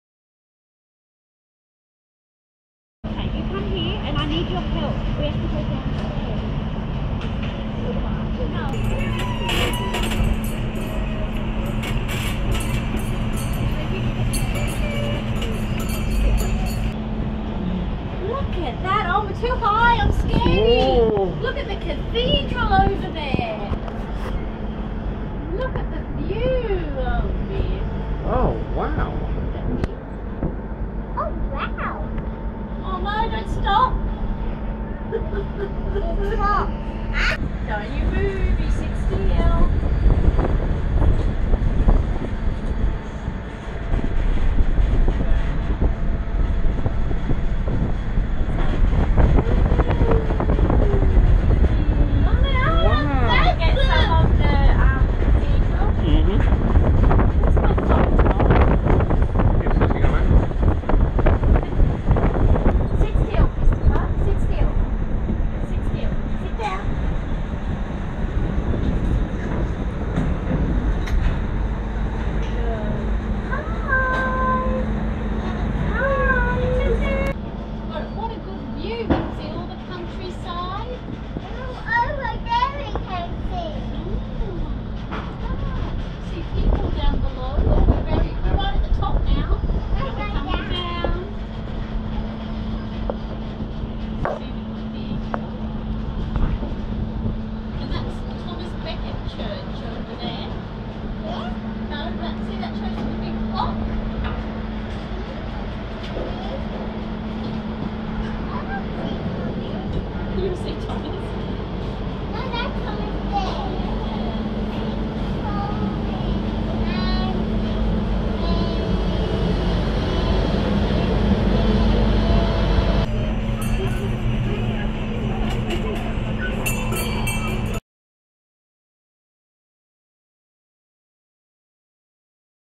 Salisbury from the air in 4k
Salisbury in Wiltshire, UK, from the air, viewed from the Salisbury Eye, August 2016. In 4k video.